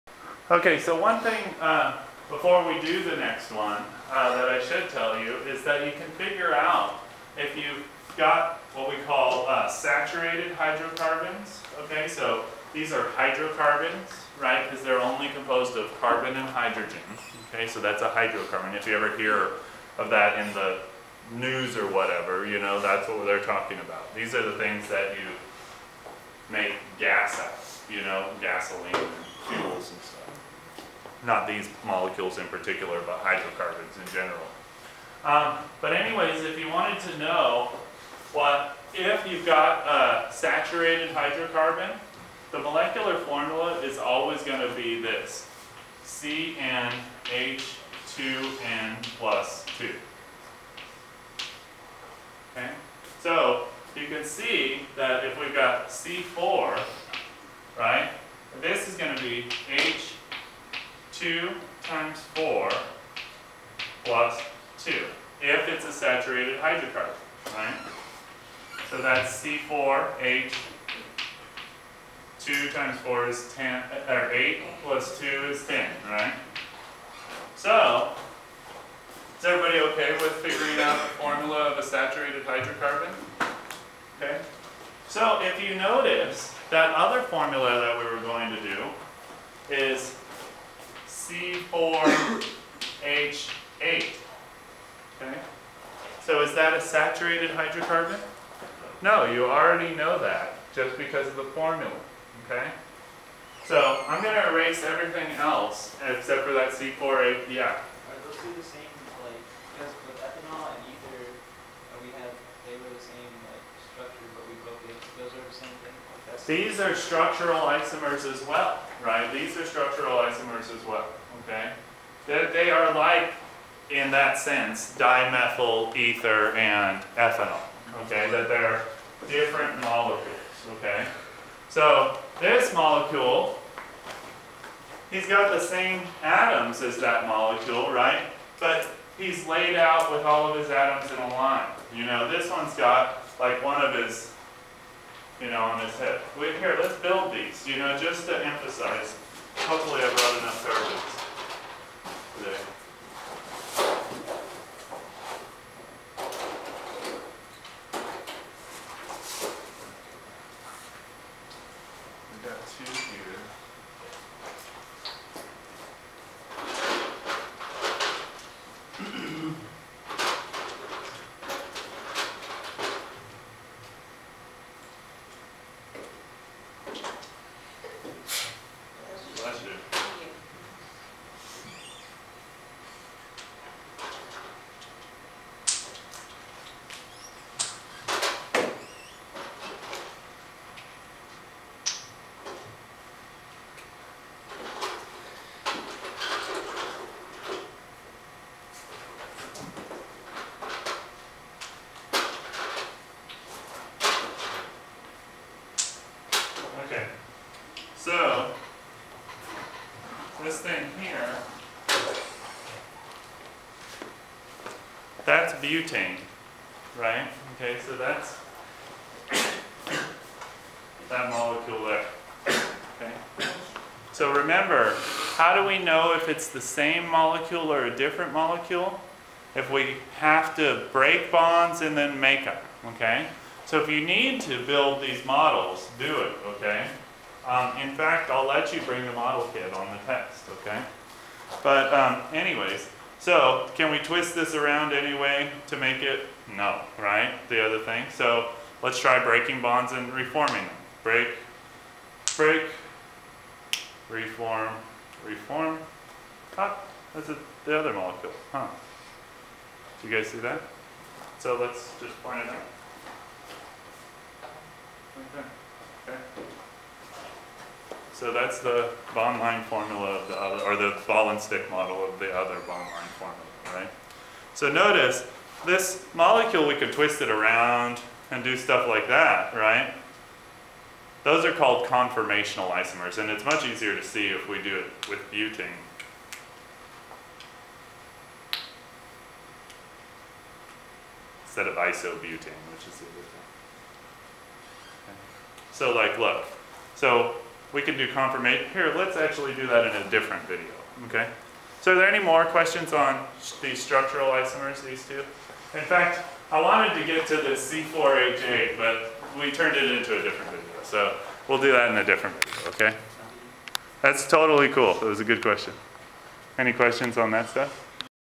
Introduction to Organic - Saturated Hydrocarbons - Structural Isomers 004
Show that the molecular formula (C4H10) is indeed a saturated hydrocarbon: CnH(2n+2)